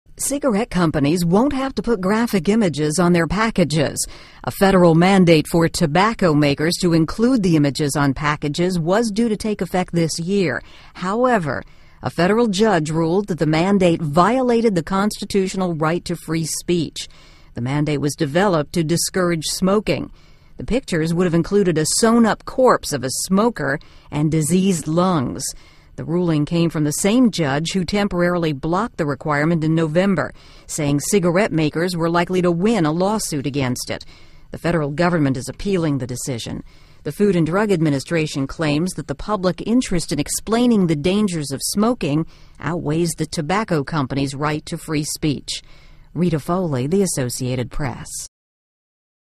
Judge Blocks Graphic Images on Cigarette Packs
A judge blocked a federal requirement that would have forced U.S. tobacco companies to put large graphic images on their cigarette packages. Warning: Contains graphic images. (March 1)